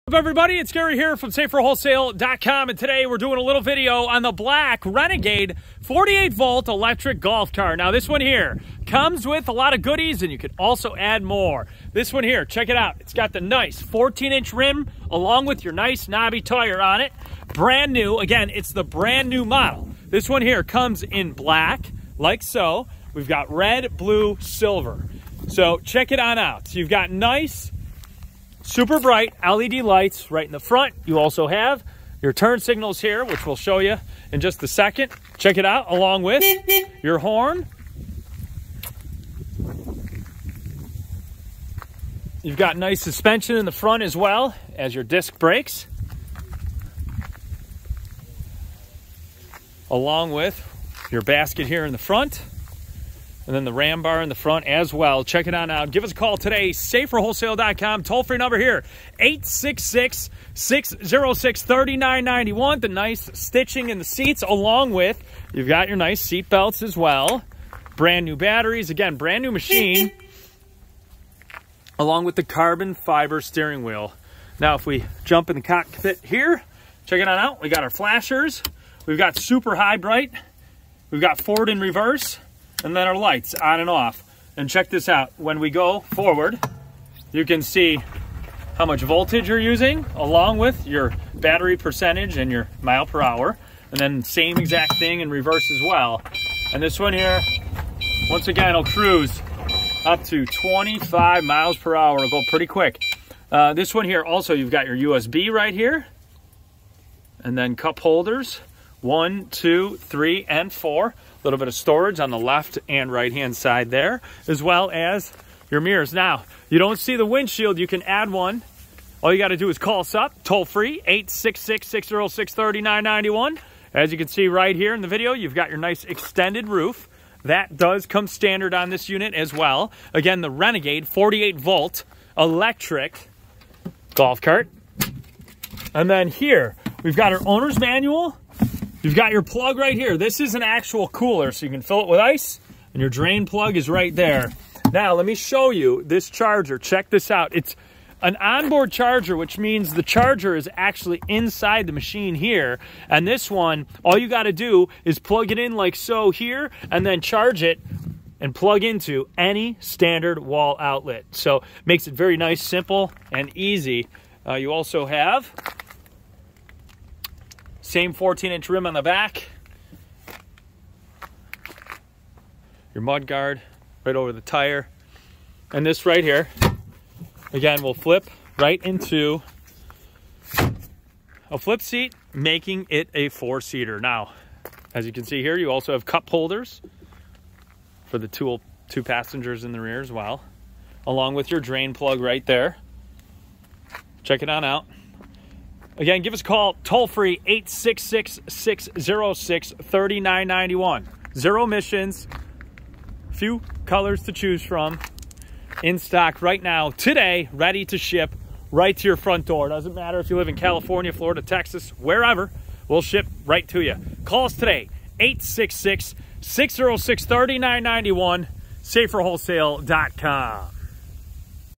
Black Renegade Electric 48v Golf Cart Review And Test Drive Monster Utility Vehicle
Check Out The 48v Electric Renegade Golf Cart In Stock Now With Turn Signals, Horn, Windshield, Lights, And Much More!